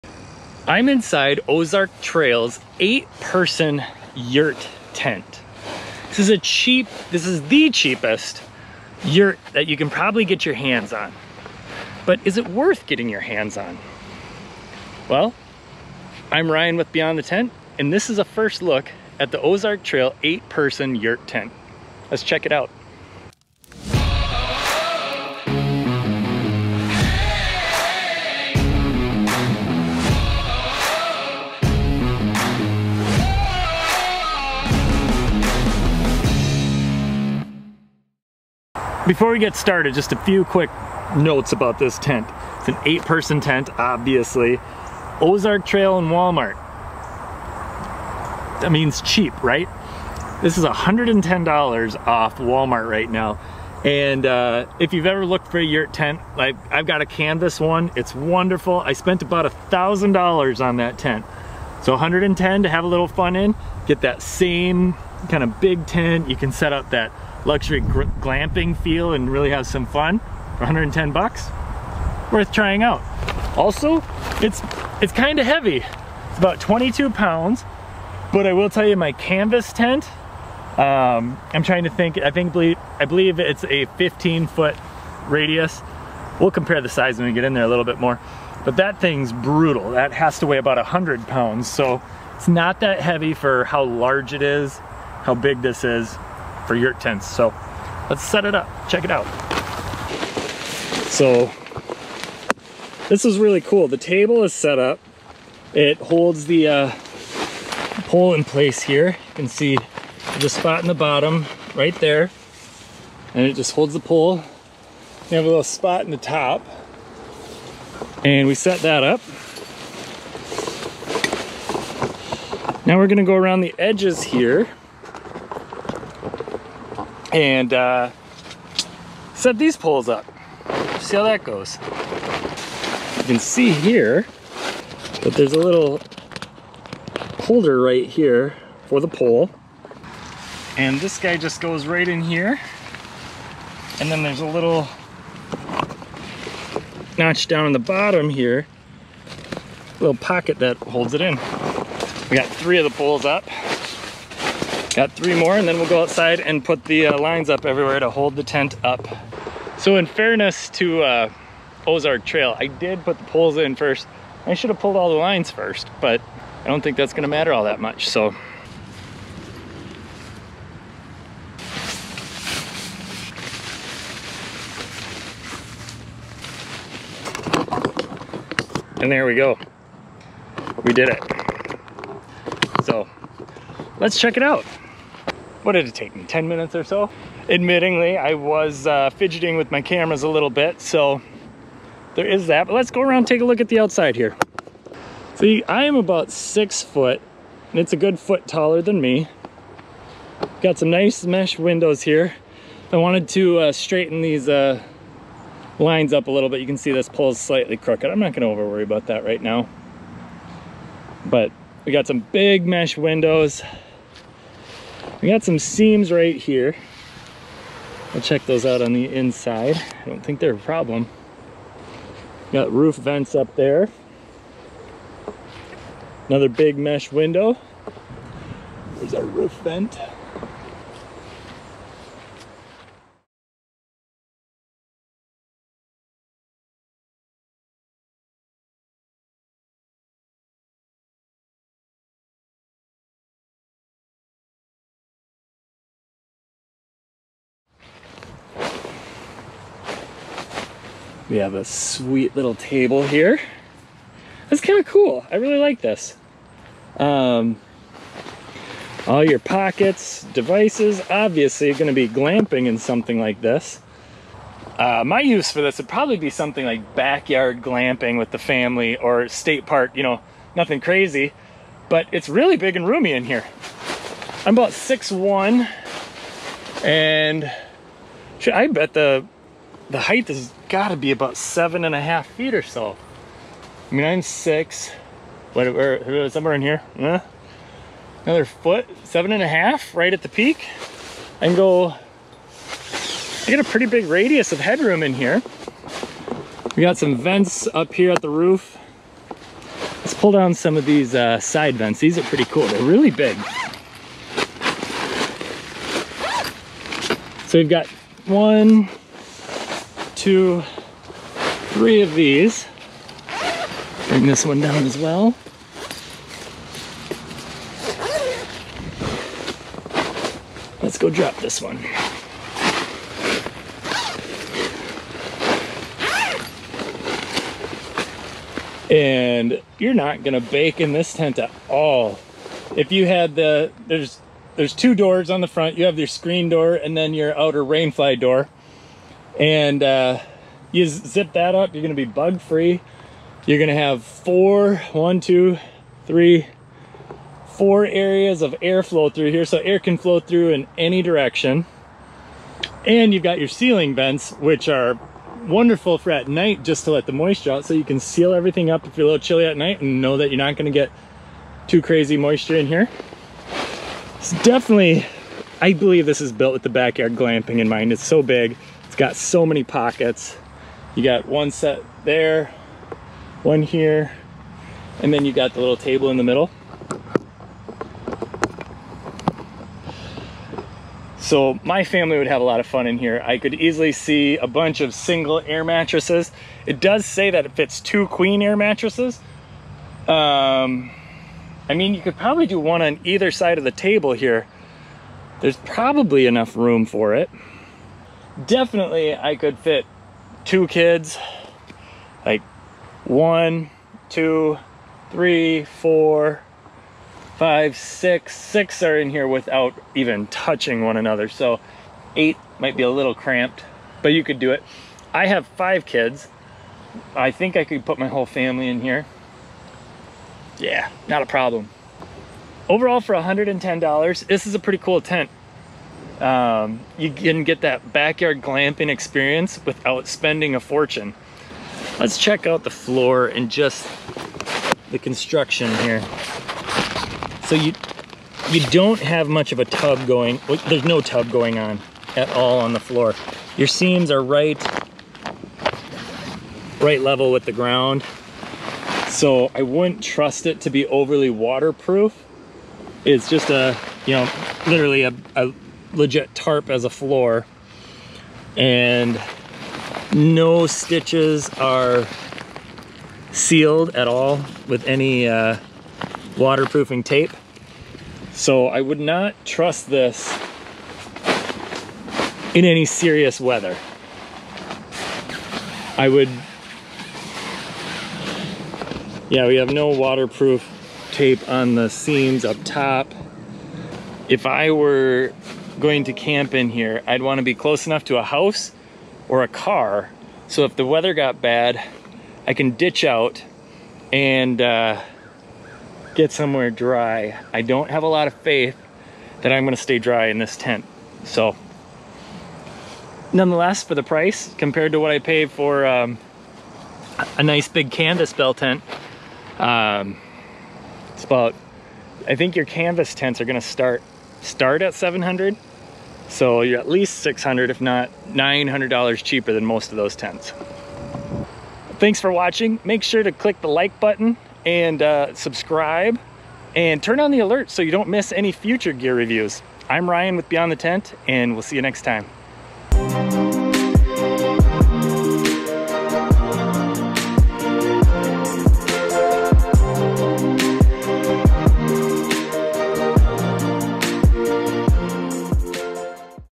The Cheapest Yurt Tent I Can Find! 8 Person Ozark Trail Yurt Tent From Walmart
This Yurt Tent from Ozark Trail (Walmart) is the cheapest yurt I could find that could fit my family. I'm taking a first look at this tent to find out if it great deal or a waste of money.